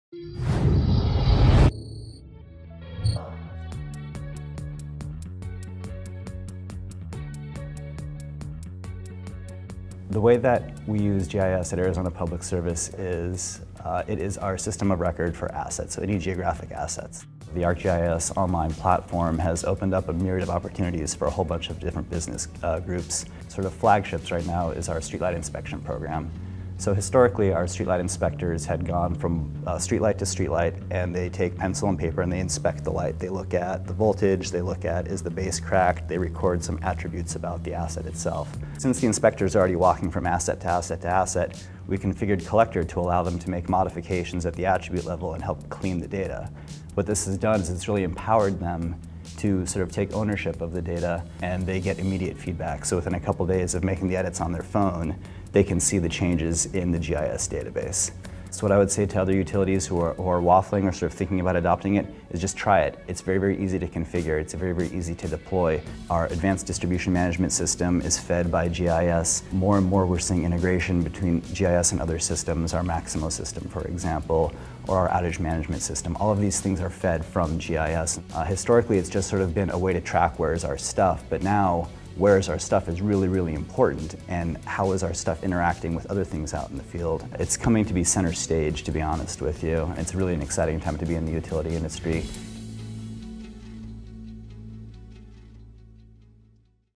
Esri UC 2017: Joshua Schwartz, Arizona Public Service
Arizona Public Service is the largest electrical provider in the state of Arizona. Using tools like Collector for ArcGIS to inspect electric poles has reduced thousands of paper maps and thousands of hours of administrative work for Arizona Public Service. This is just one example of GIS being used to conduct more efficient and complex analyses within every department of Arizona Public Service.